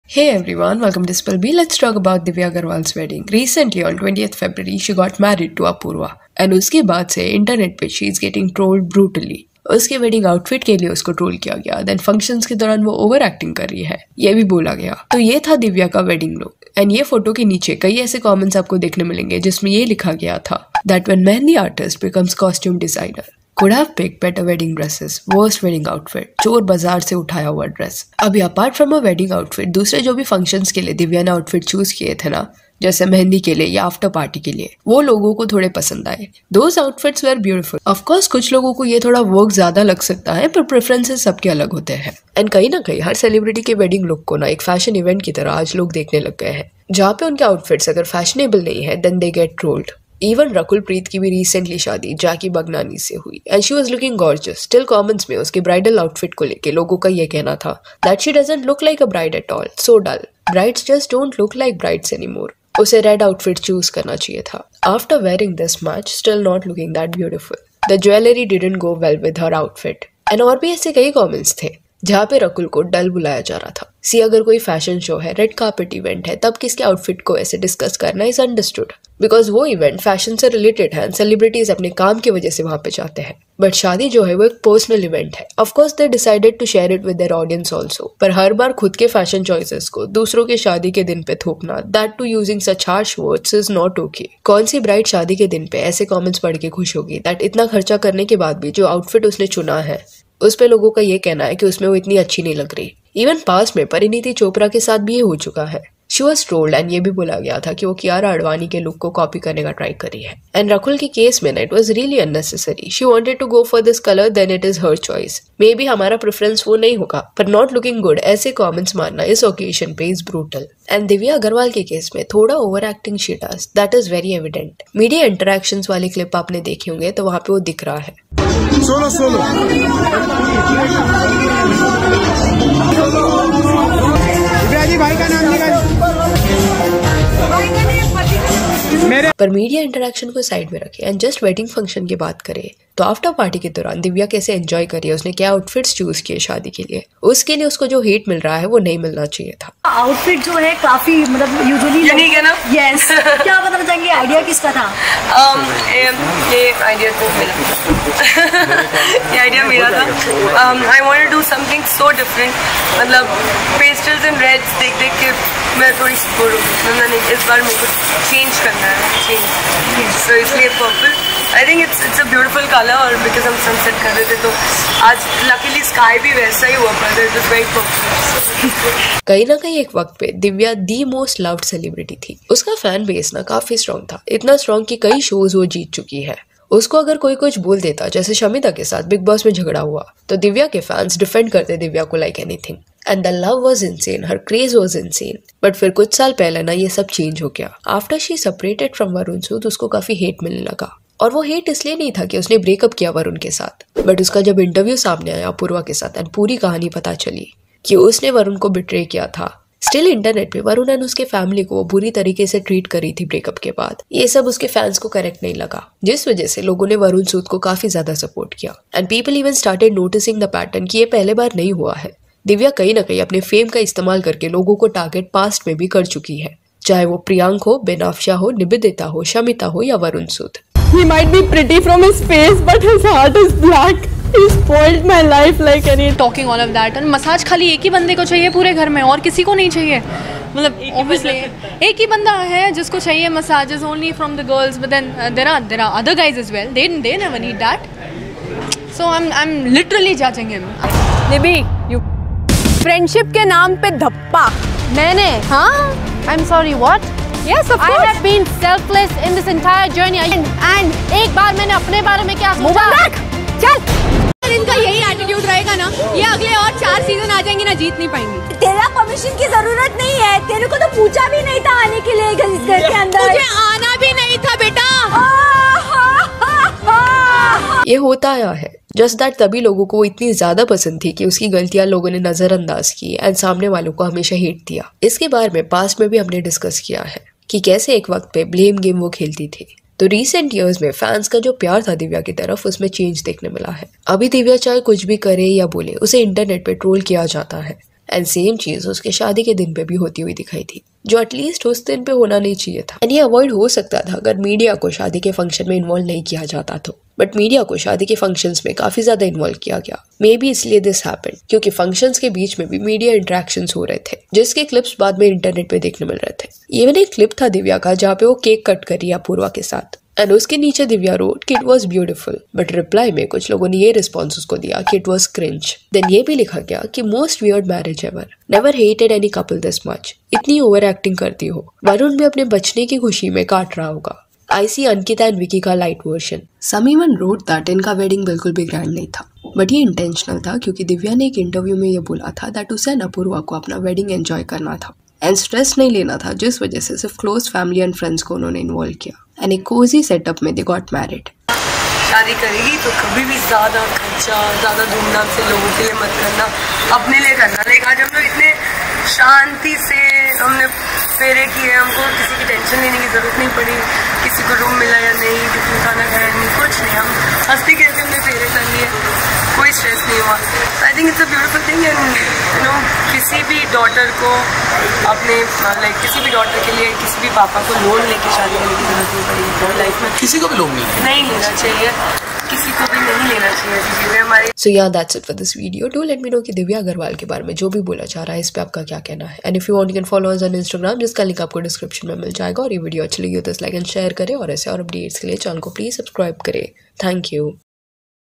DIVYA AGARWAL & APURVA PADGAONKAR'S WEIRD WEDDING OUTFIT | RAKUL PREET COPYING KIARA ADVANI
About this video: In this video, we will talk about Divya Agarwal, Apurva Padgaonkar, Rakul Preet and Jackky Bhagnani